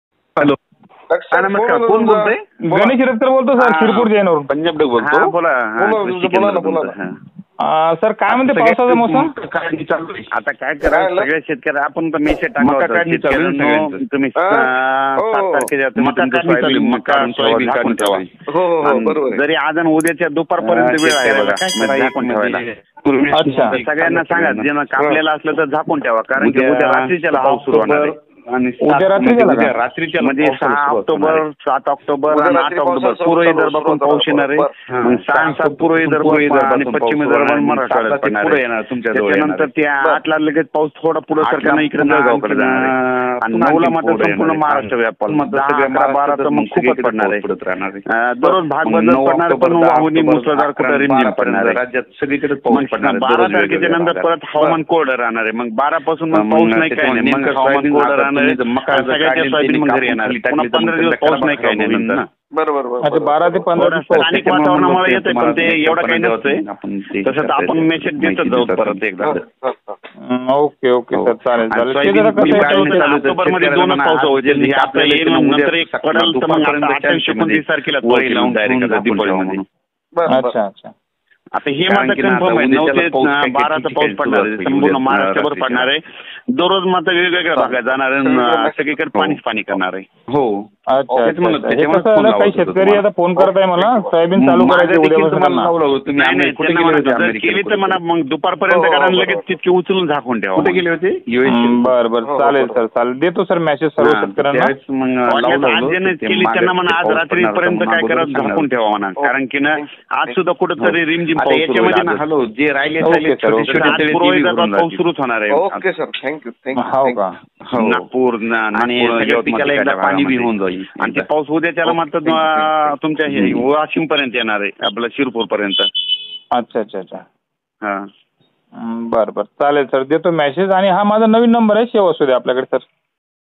पुढील तिन दिवसांत धो धो पाऊस पडणार
पंजाबराव डख गुगळी धामणगाव प्रसिद्ध हवामान अभ्यासक यांचा नविन अंदाज आला